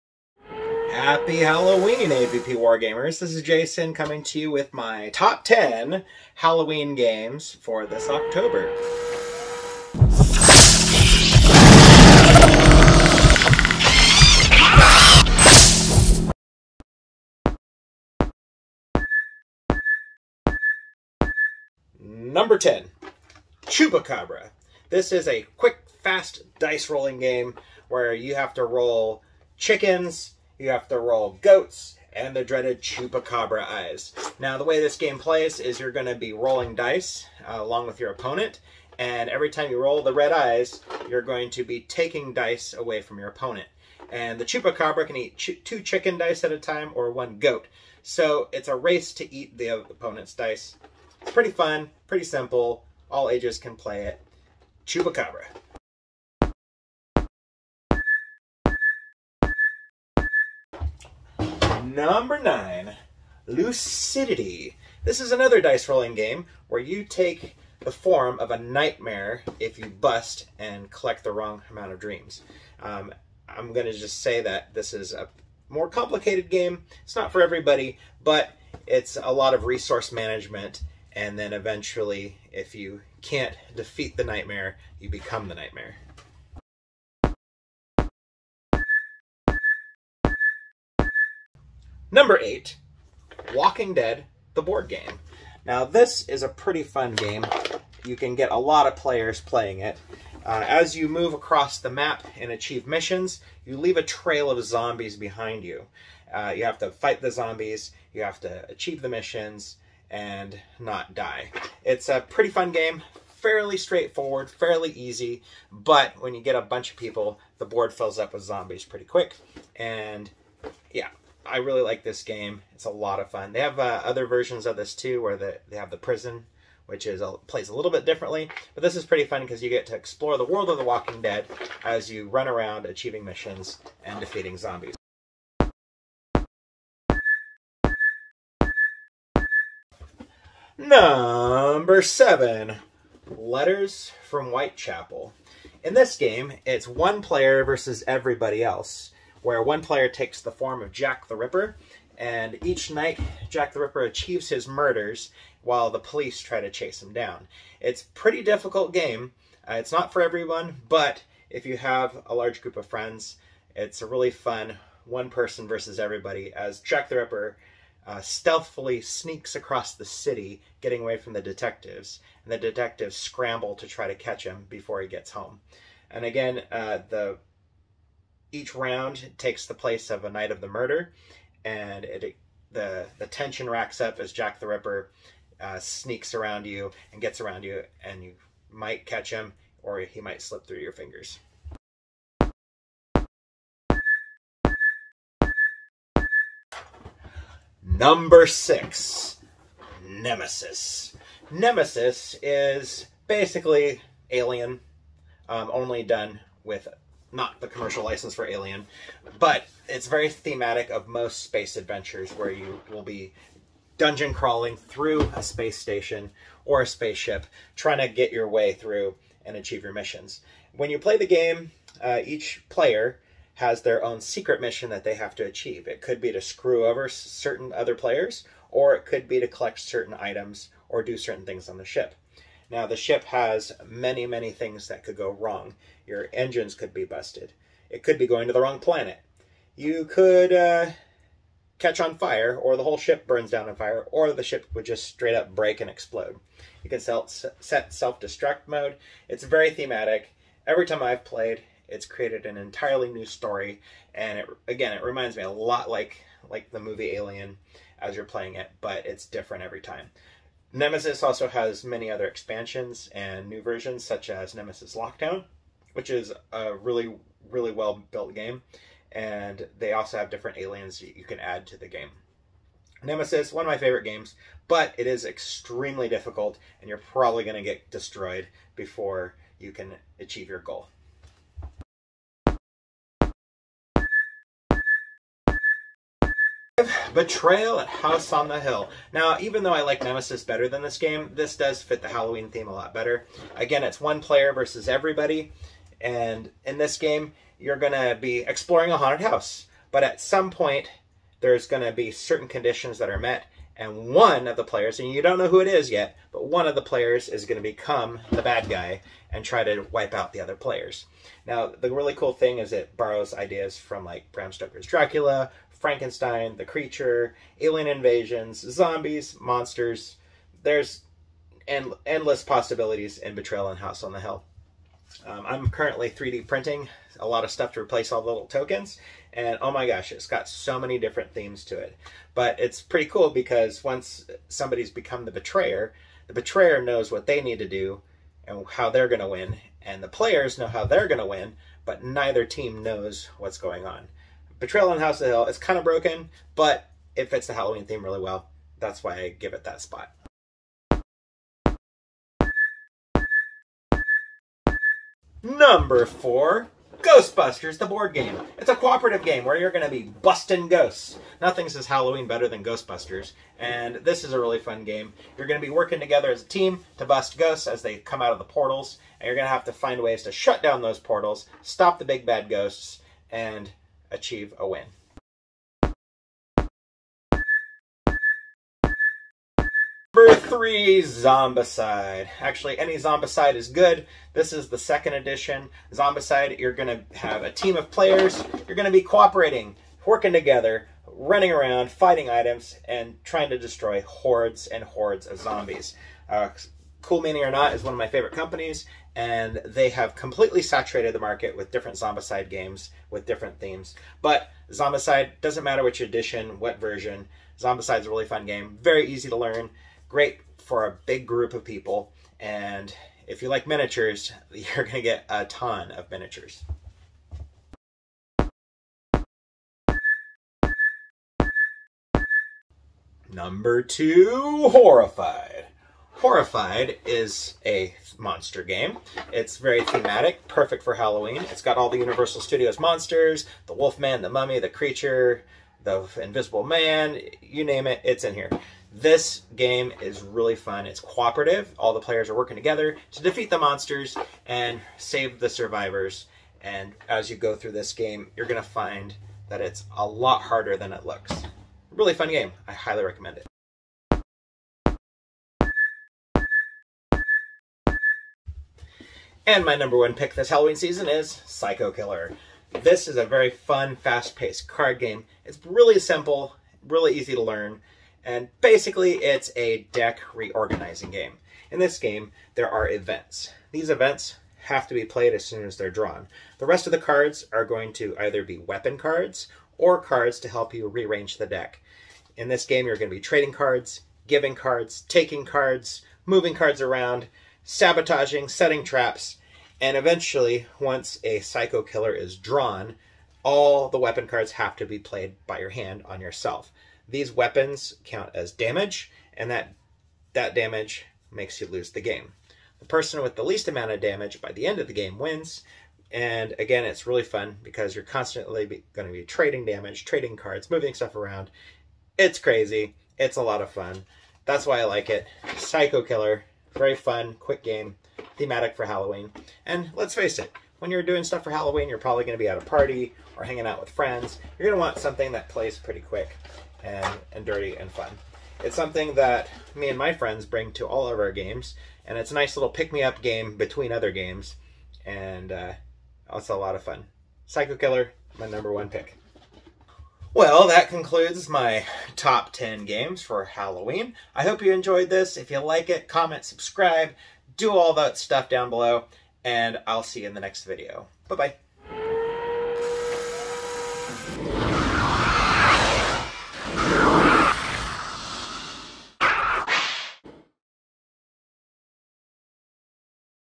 Top 10 Halloween Games 2022
# 5 Betrayal at house on the hill BUY
#8 Walking Dead the board game (Prison version)
#10 Chupacabra (Not sure where to buy, try ebay)

Enjoy! Join My Facebook Group AVP WARGAMER